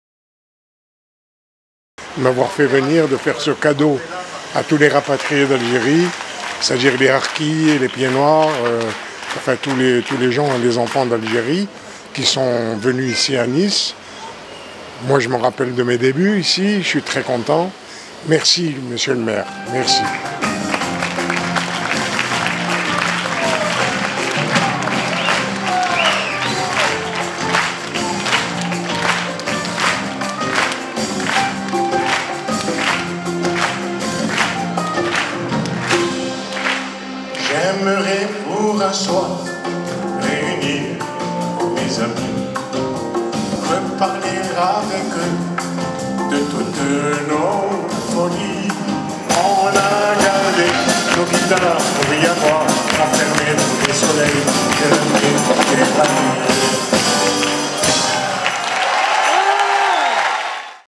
Concert exceptionnel d’Enrico Macias
Ce dimanche 22 septembre 2019 devait se tenir la 10ème édition de « Au Soleil des deux rives », un événement organisé par la Ville de Nice avec le concours des associations de Harkis et Pieds-Noirs au Jardin des Arènes de Cimiez. En raison des intempéries annoncées, la manifestation est reportée au 20 octobre prochain. Mais le concert d’Enrico Macias, programmé pour cette édition anniversaire a eu lieu au Palais de la Méditerranée.